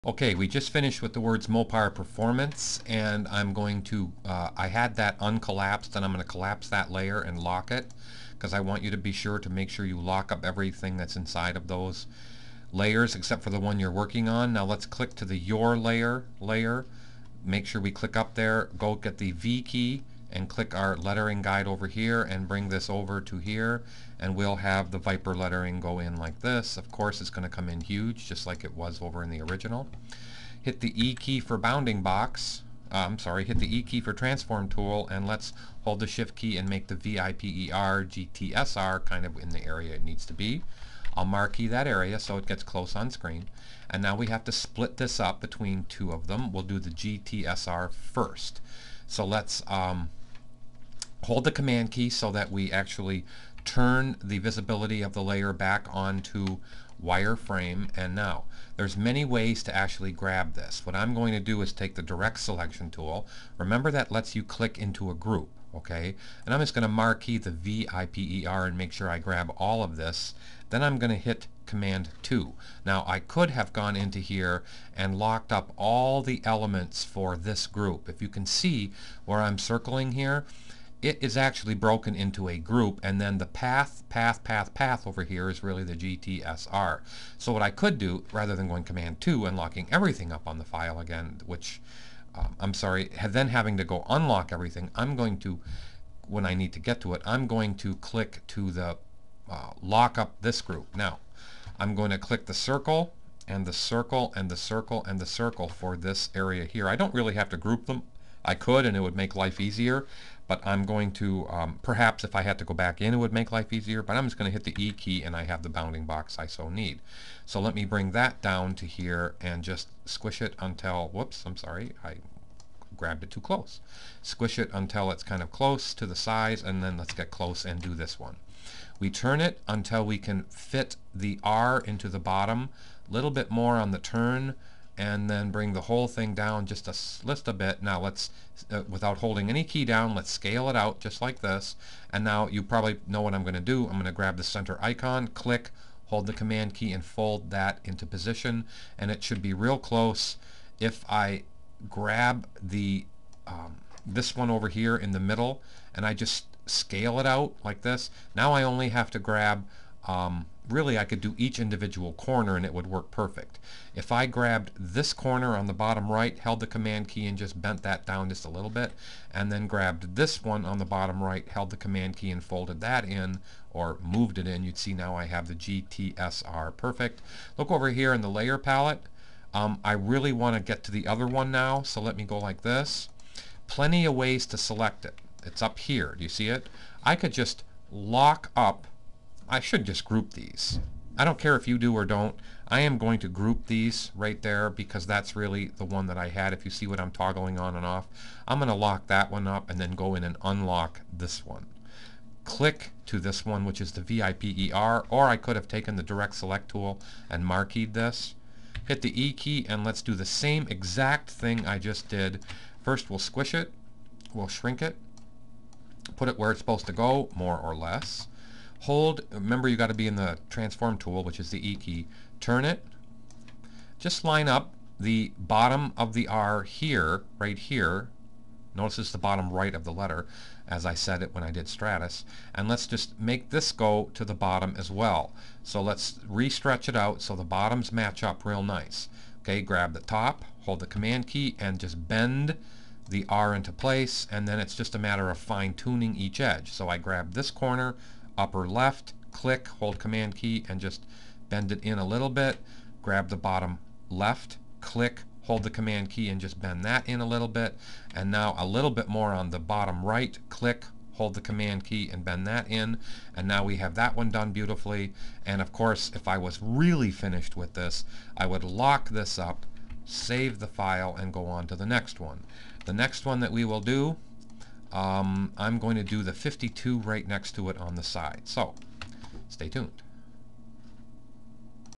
1200 Guide 5 • Transform Tool Tutorial Movie 3 • GTSR
This is the 3rd of 10 short tutorial movies that will show you how to bend and distort lettering shapes onto existing artwork.